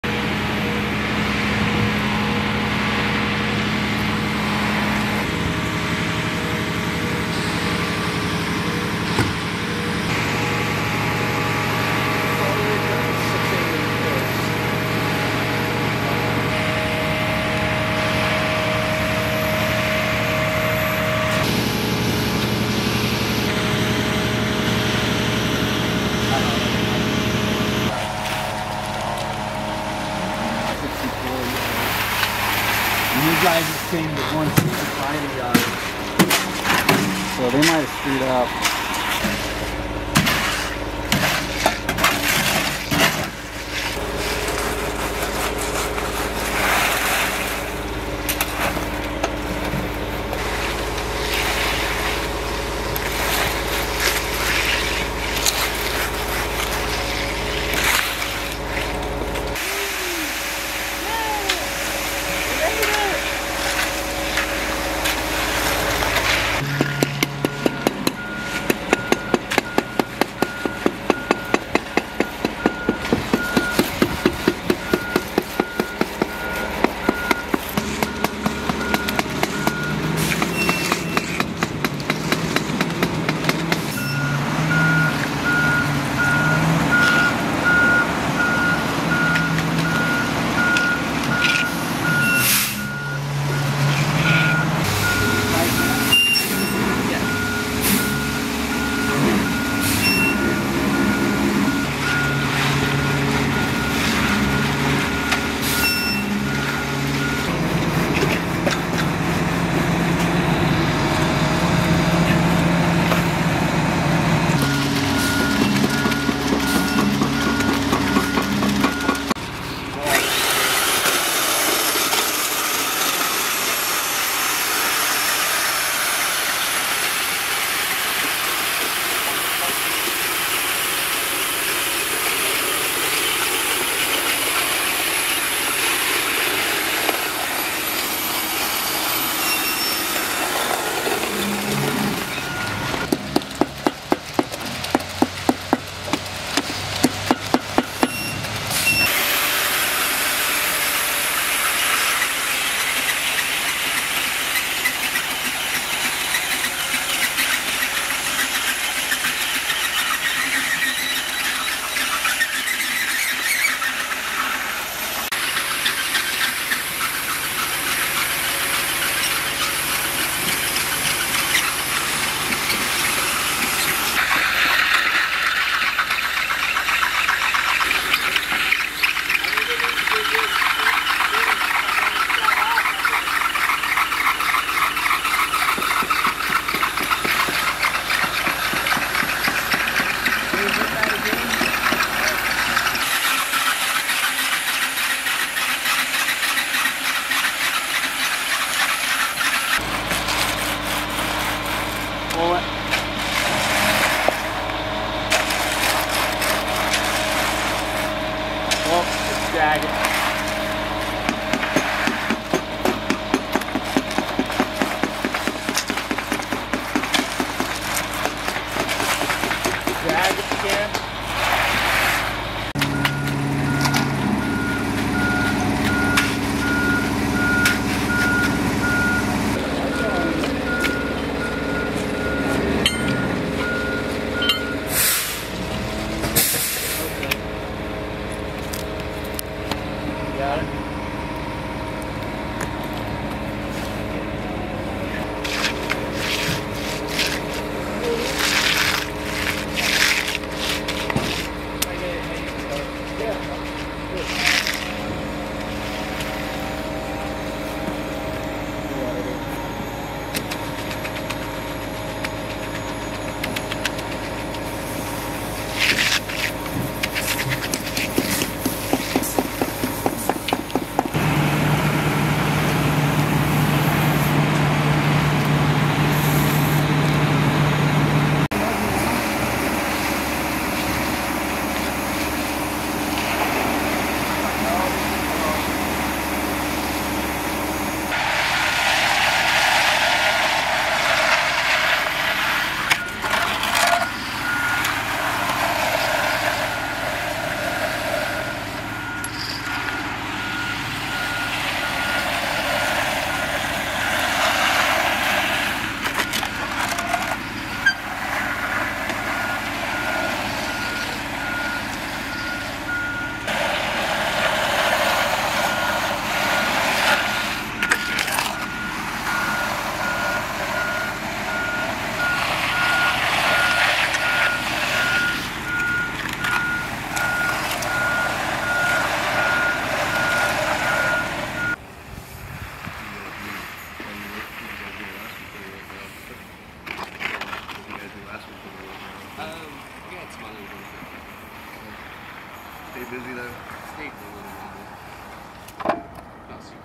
Hand Screeding Concrete Vs. Machine Screeding Concrete
In this video we show the difference between hand screeding and machine screeding concrete. How do you like to screed concrete and have you ever used a roller screed?

Also Buy From